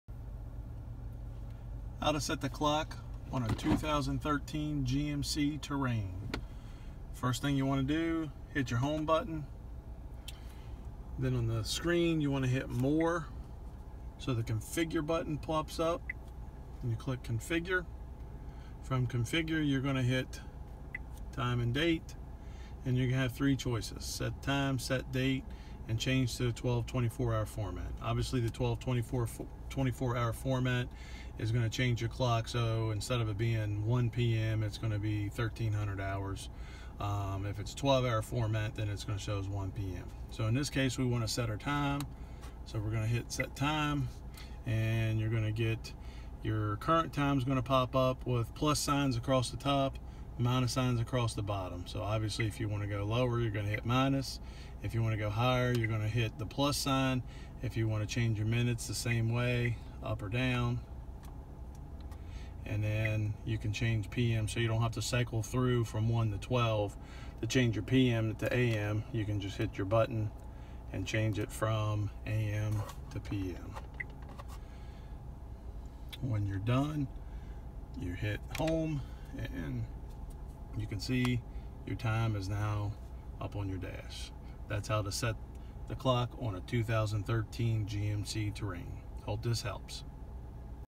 How to set the clock on a 2013 GMC Terrain
This video will help you set the clock on a 2013 GMC Terrain. We also love comments.